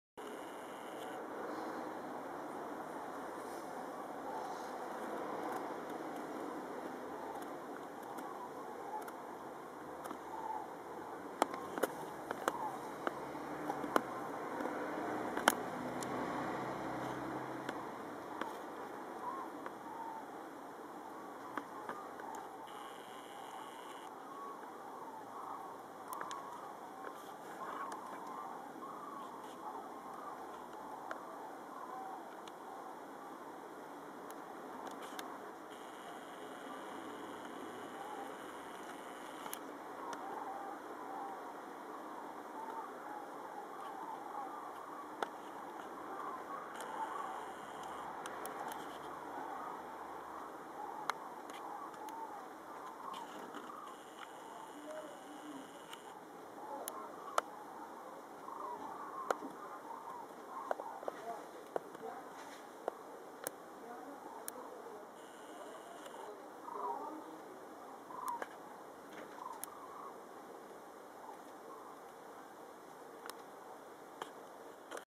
Zugvögel wieder zurück
kurz gekreist und weiter richtung Norden...